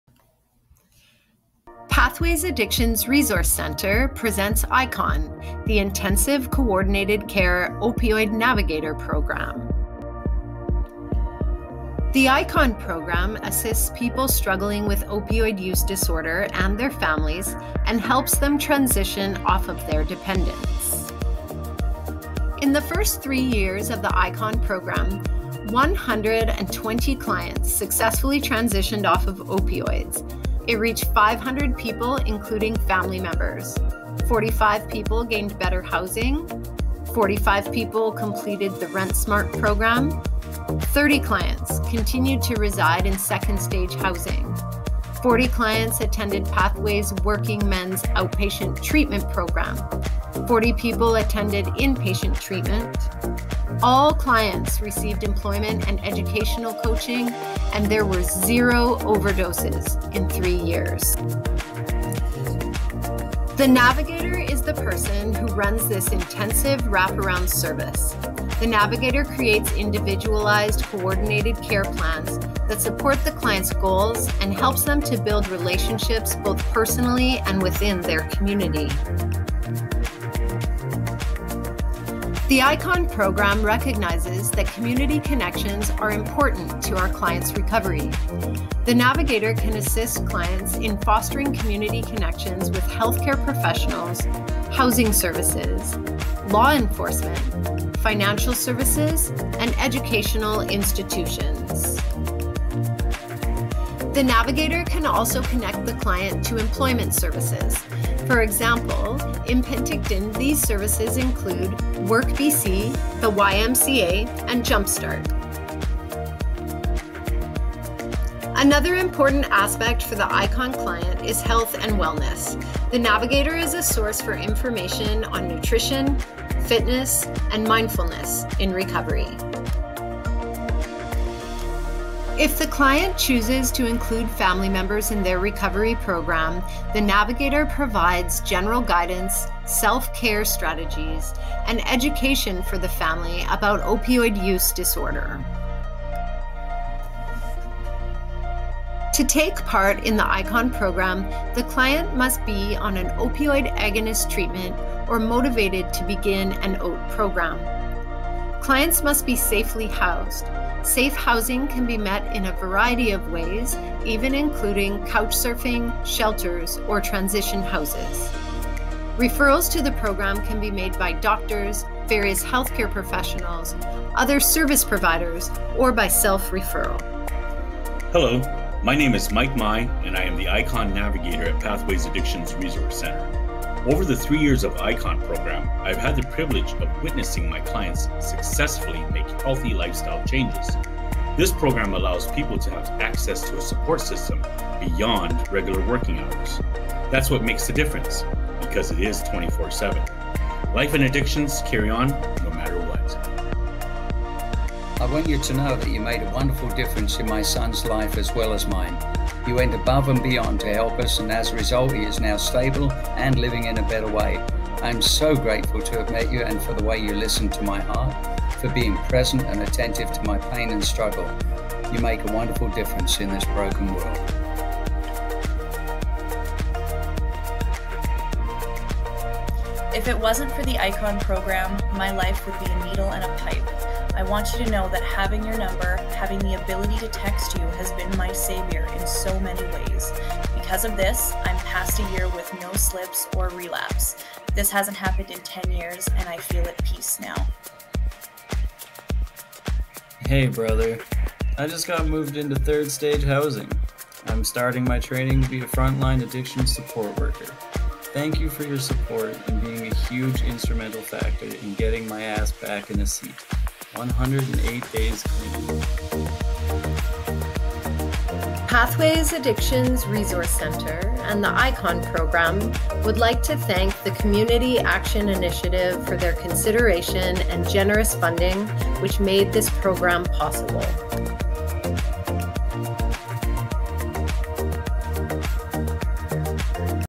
Pathways Addictions Resource Centre - ICCON Program
The Intensive Coordinated-Care Opioid Navigator (ICCON) program assists people struggling with opioid use disorder and helps them transition off their dependence. Pathways Addictions Resource Centre is located in Penticton, BC, Canada.